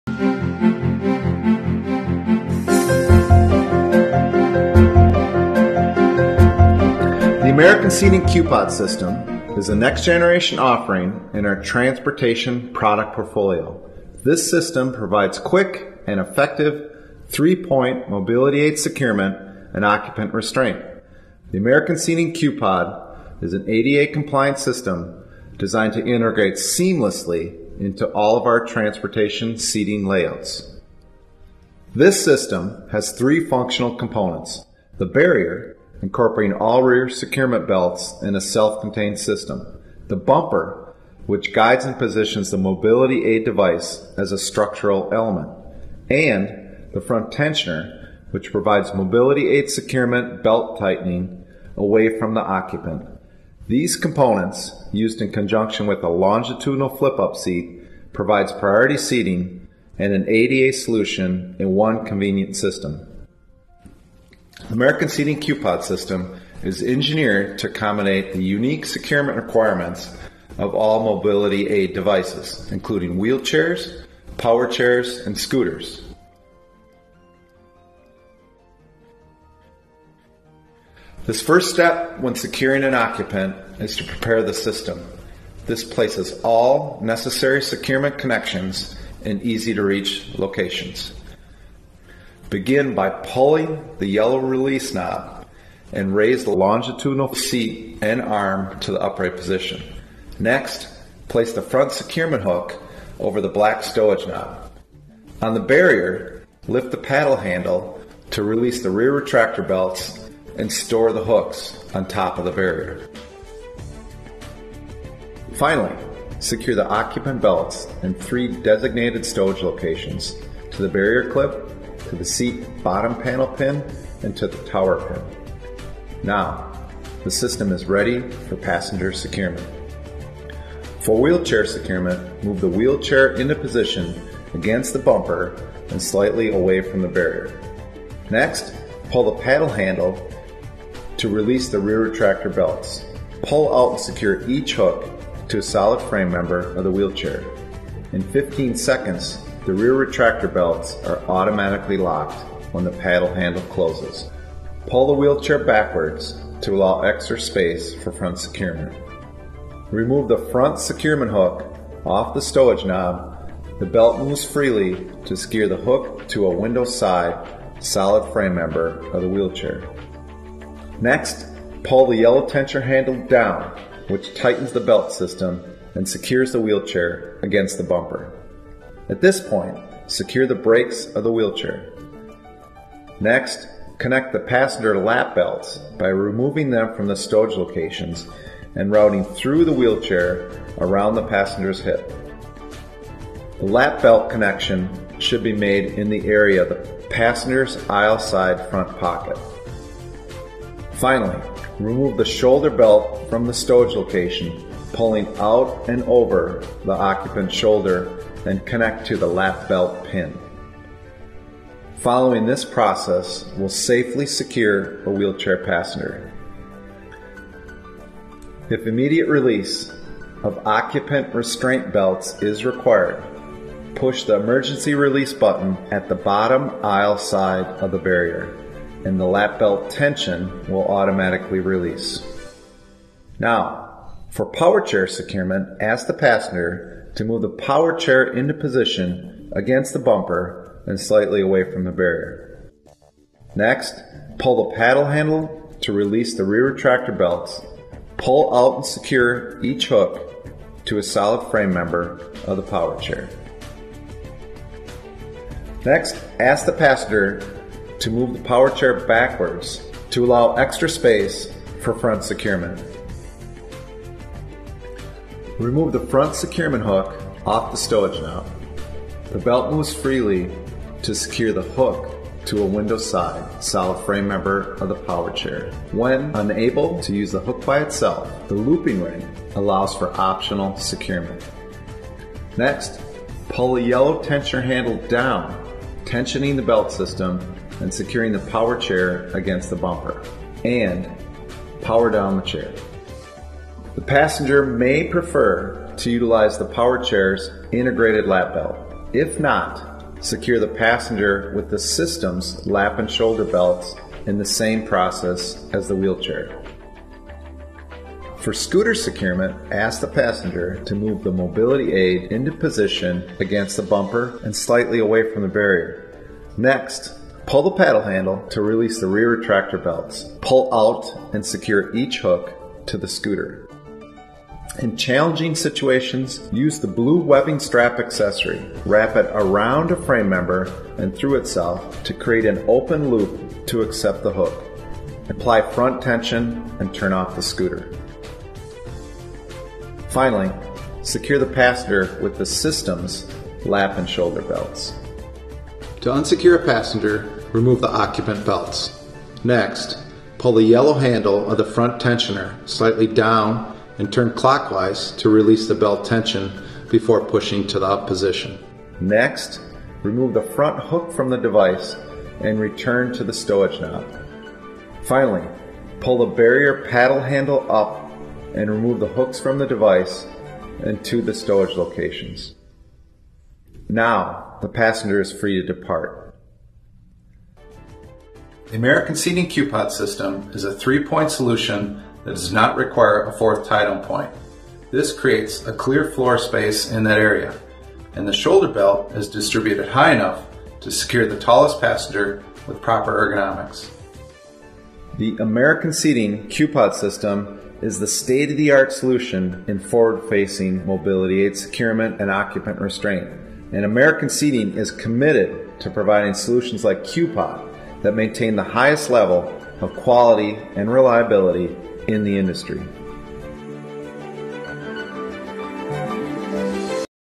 American Seating Q'POD® Training Video
The Q’POD is the next generation offering in the American Seating A.D.A. portfolio. This system provides quick and effective 3-point mobility aid securement and occupant restraint.